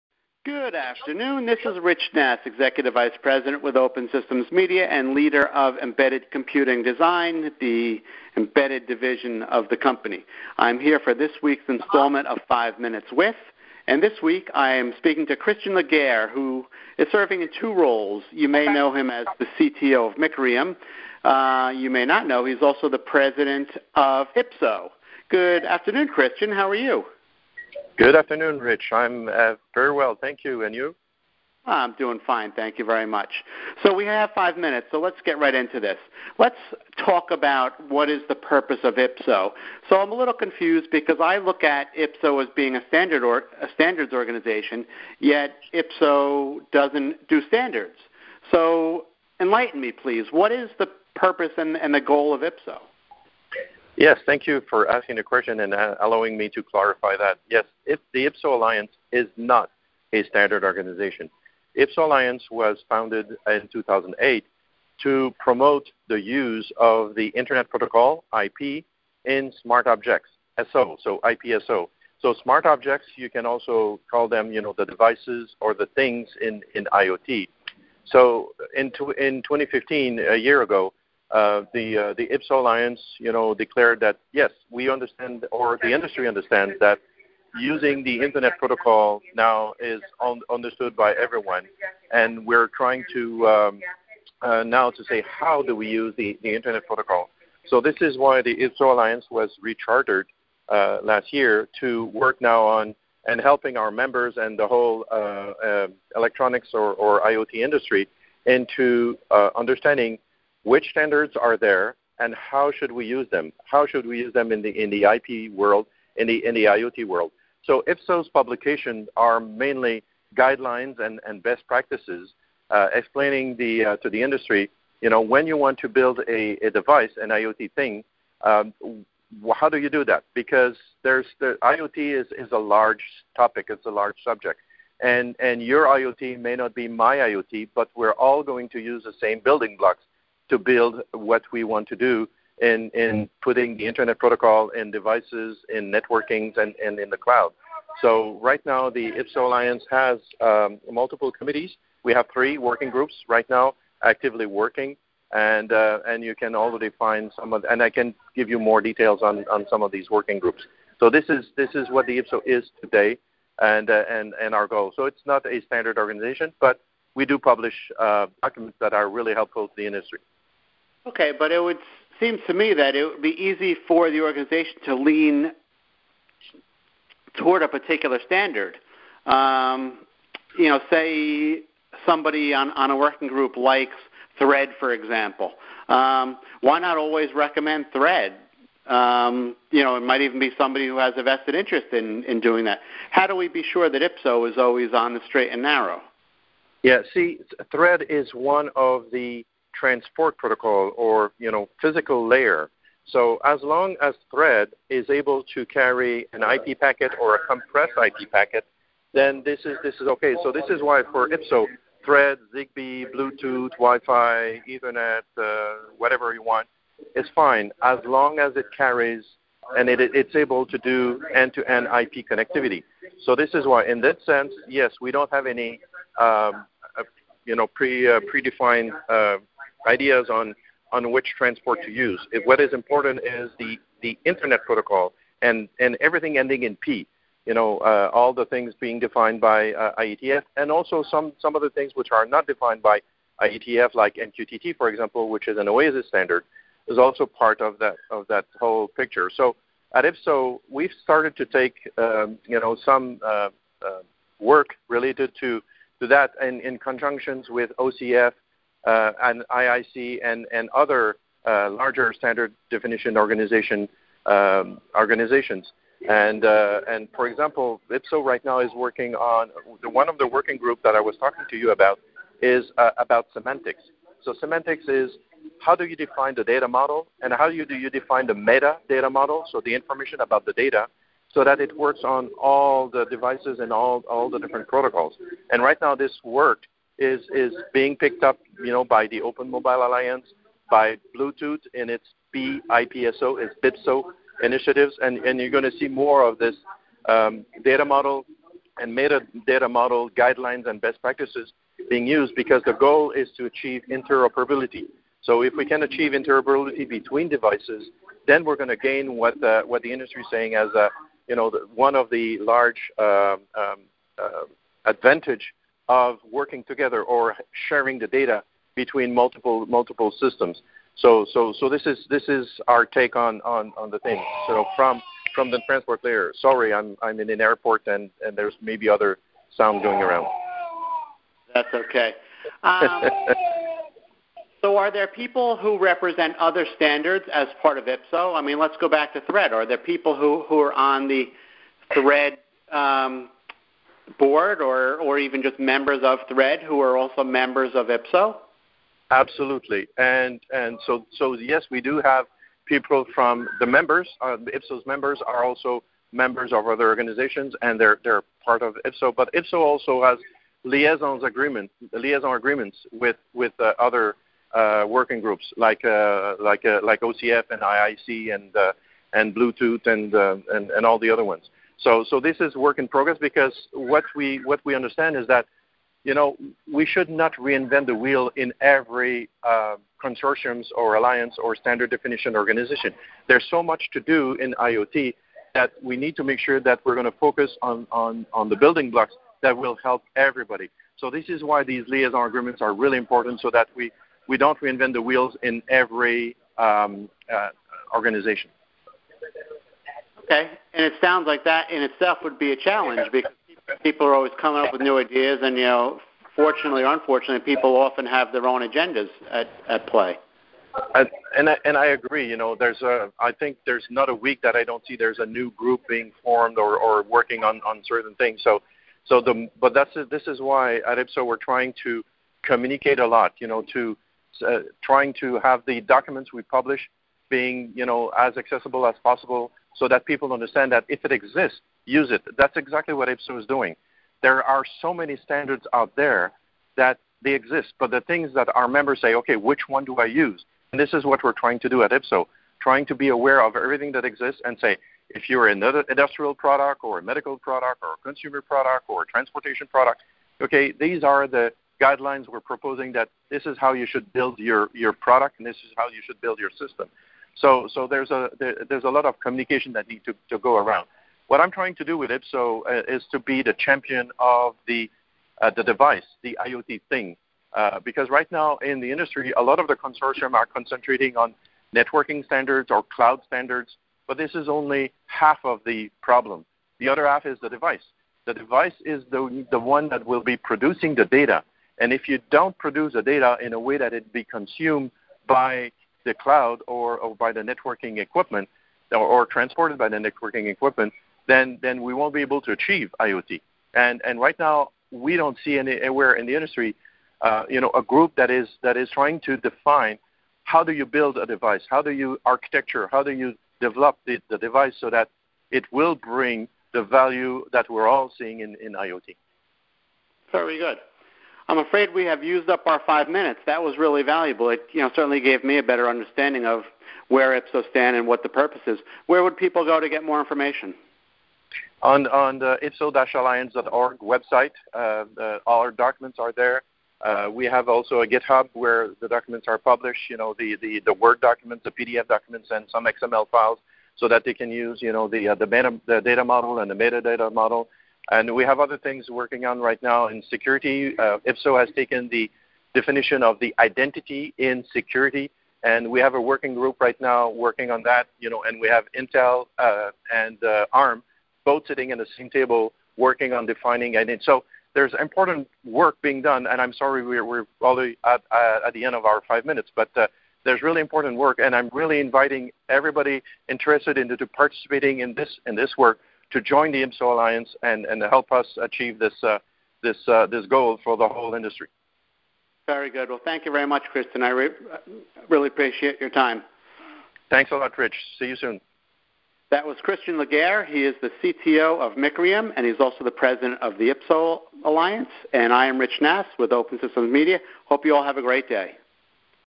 Five Minutes With…Christian Legare, President, IPSO Alliance
IPSO is a standards organization that doesn’t do standards. Huh? Well, that’s sort of accurate. This week, I spoke to Christian Legare, who is the President and Chairman of the IPSO Alliance, and he was able to put it into a context for me that made sense. It’s all about protocols and technologies that work seamlessly. We discussed some standards in general, or others specifically, like Thread, and Christian explained how IPSO plays a role in identifying the various components that a designer needs to be concerned with.